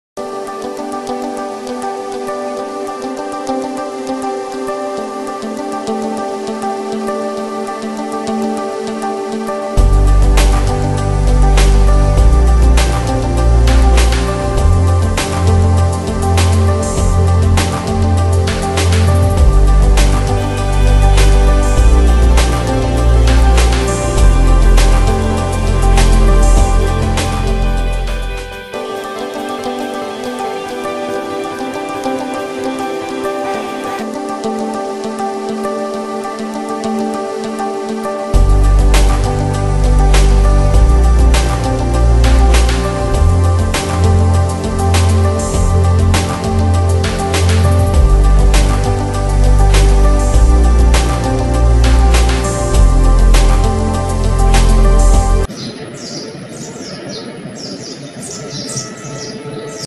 Flowering in Elephant's tooth cactus# shorts
Coryphantha elephantidens commonly known as Elephant's Tooth, Starry Ball is native to Mexico. It is a succlent belonging to family Cactaceae of order Caryophyllales. Large, stout and broad-based tubercles with groove & scarce spines growing only radially in the areole characterize this plant. Flowers grow from thebase of young tubercles, which arise from the tip of the plant.